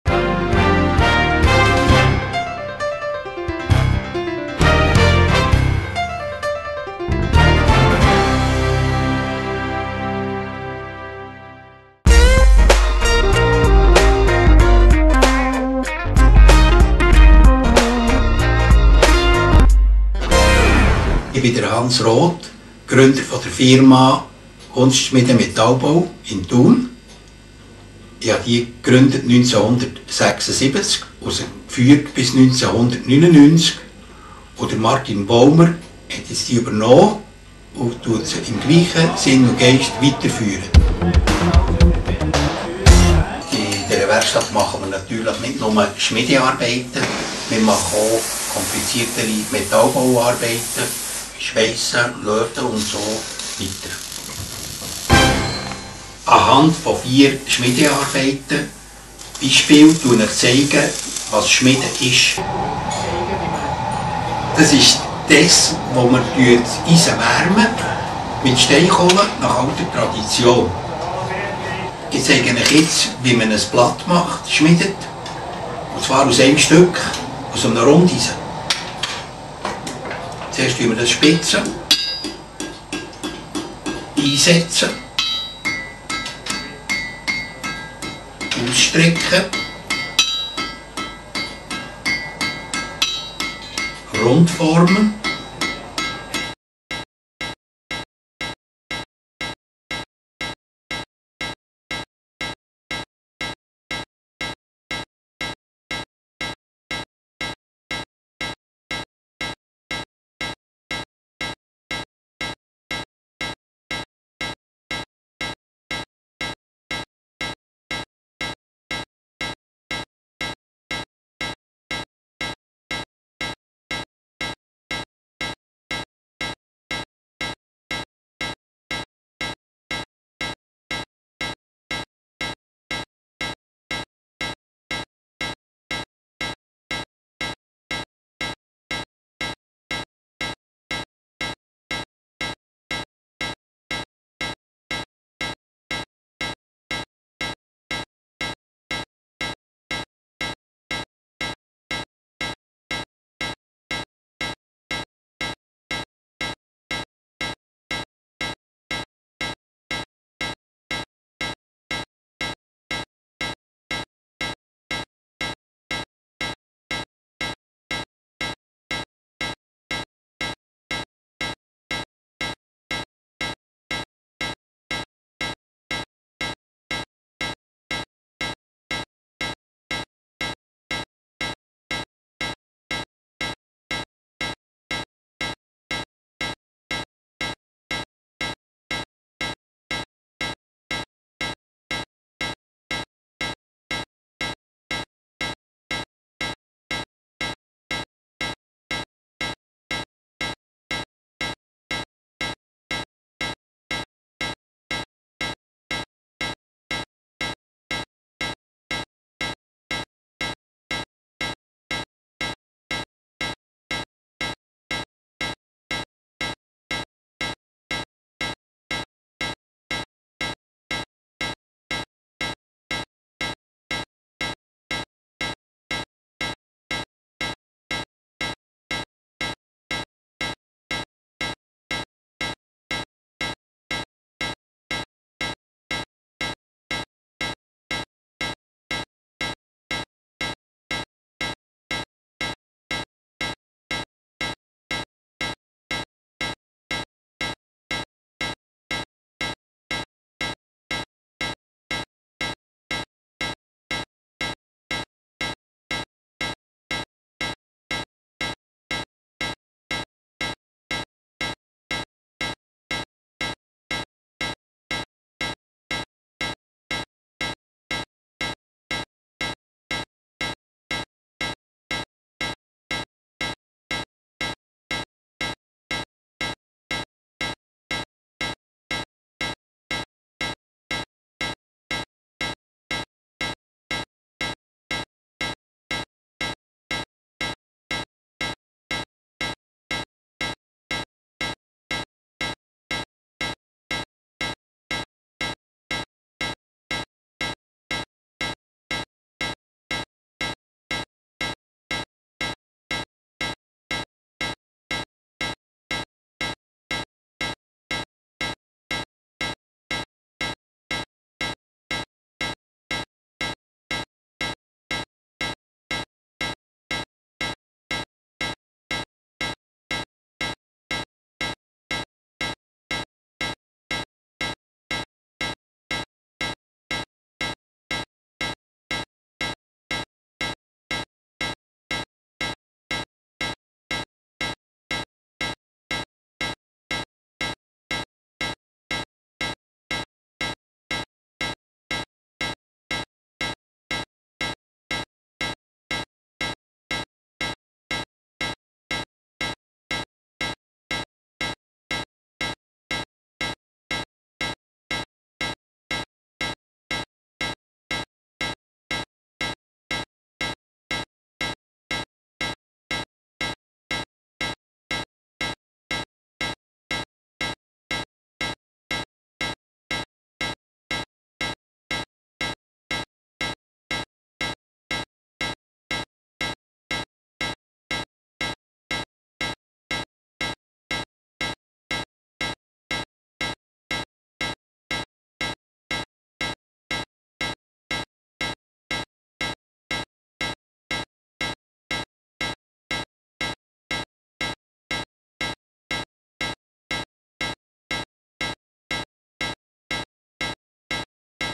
Kunstschmiede Roth GmbH Thun (Schweiz) Hans Roth
Hans Roth an einer Schmiede-Demo